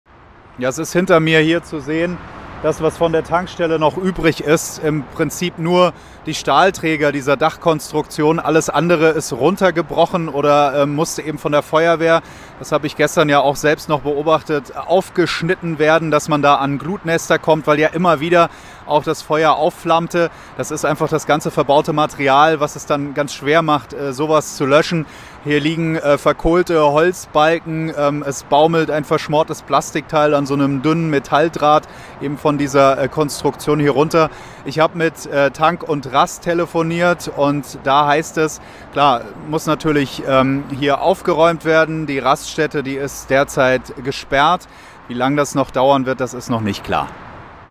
Auto rast in Tankstelle - der Tag danach
Der Tag nach dem Flammen-Inferno an der A5 in Südhessen: Nur noch ein Gerippe ist von der Tankanlage Gräfenhausen-Ost zu sehen. Bis zum Abend hatten hier bis zu 180 Feuerwehrleute gegen die Flammen gekämpft. FFHReporter Benjamin May ist vor Ort